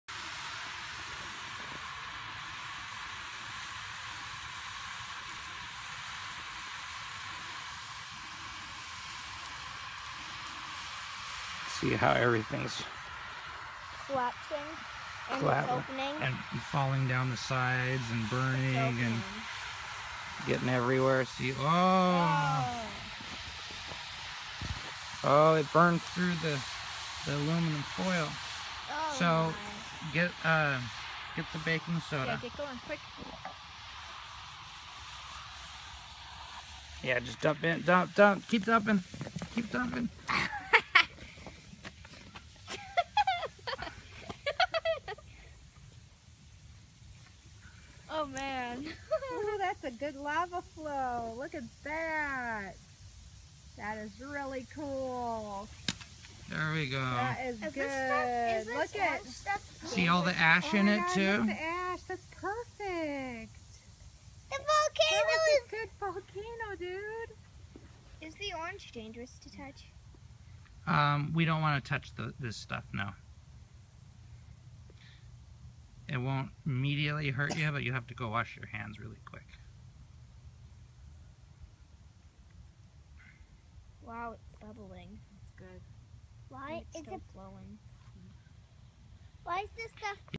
Science Fair Volcano 'Plus'
We first cause an explosion driving ash everywhere using Ammonium Dichromate and follow it off with the traditional baking soda and vinegar lava flow.

(our baking soda to vinegar ratio was correct but we had too much vinegar in the volcano to begin with so the result was a lot of extra baking soda)